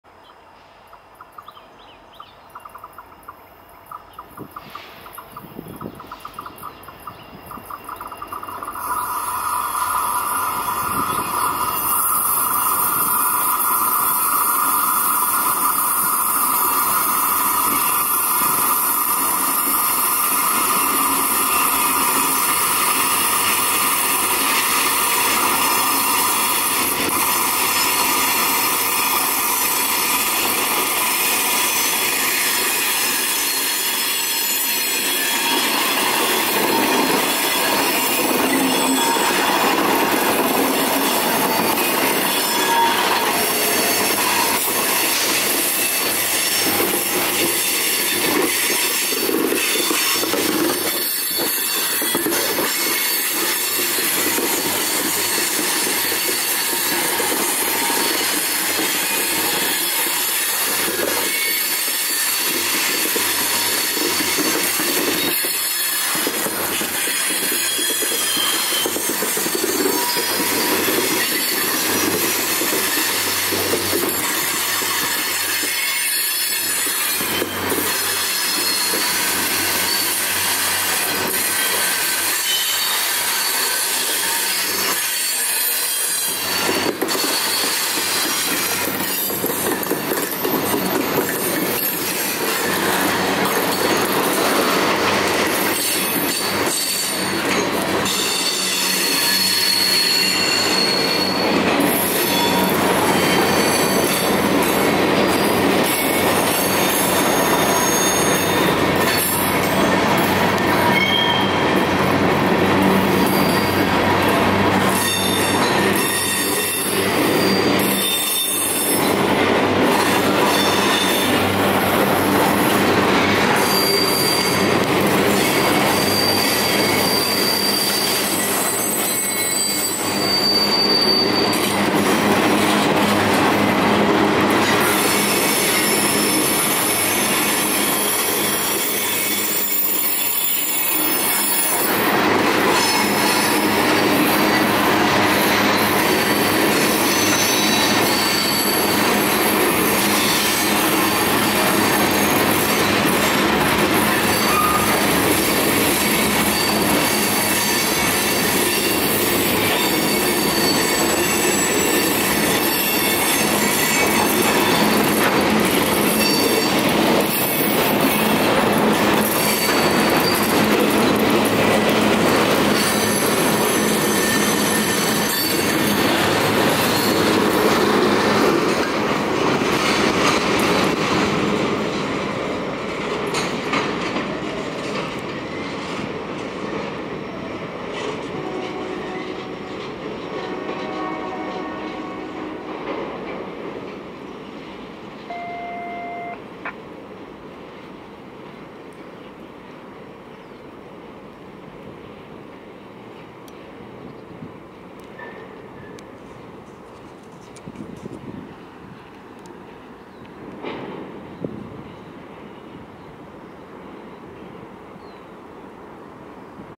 (4/1/24)UP1017 leads UP1420, UP1939 along w 40 traincars through NilesJct towards
(4/1/24) - UP1017 is on point leading UP1420 & UP1939 along with 40 traincars through NilesJct turning south towards WarmSpringsYard .